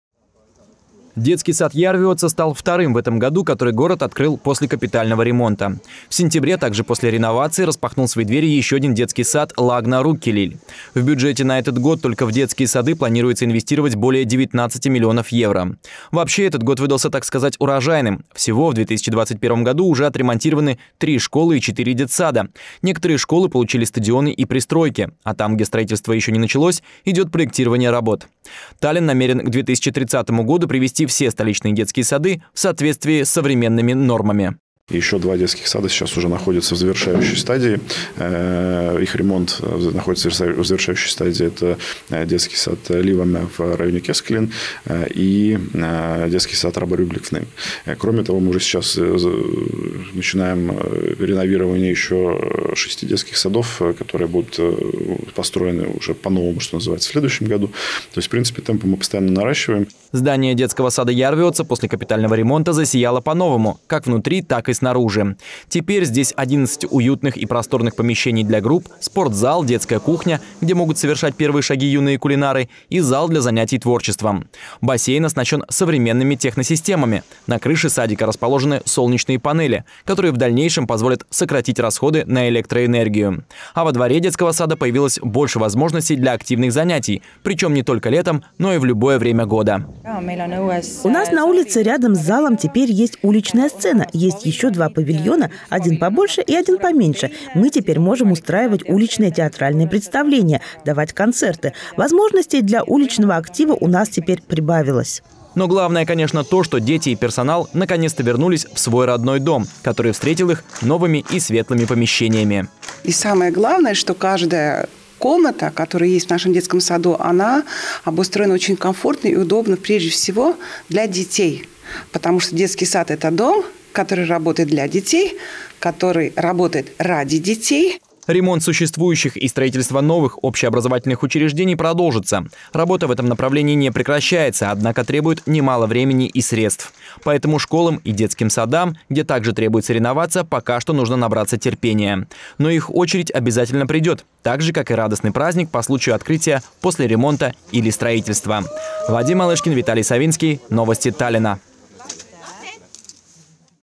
Детсад «Ярвеотса» открылся после масштабной реновации
Поставленная городом цель — привести все детские сады Таллинна в полный порядок к концу этого десятилетия — сегодня стала на один шаг ближе. После масштабной реновации сегодня состоялось торжественное открытие детского сада «Ярвеотса». Подробности — в видеосюжете «Новостей Таллинна».